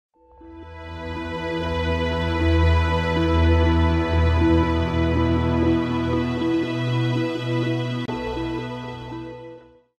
Lincoln St. Marks Time Shift
From train station to shopping centre. Who remembers heading off to London from here?